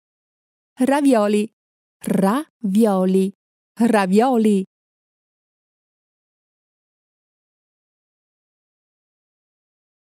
Ravioli
Ravioli are a traditional type of Italian filled pasta. They are composed of a filling sealed between two layers of thin egg pasta dough and are served either in broth or with a pasta sauce.